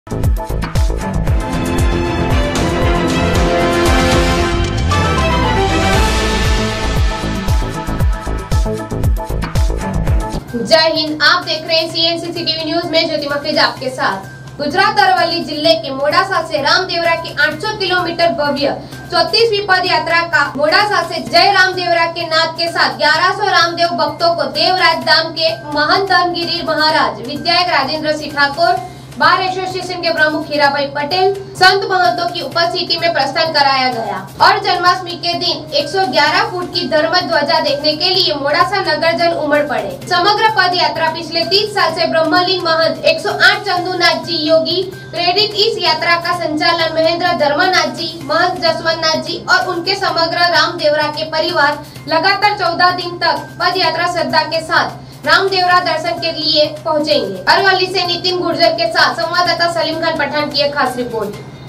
CNCC TV NEWS--अरवल्ली जिले के मोडासा से रामदेवरा की 800 किलोमीटर भव्य 34वी पदयात्रा
अरवल्ली जिले के मोडासा से रामदेवरा की 800 किलोमीटर भव्य 34वी पदयात्रा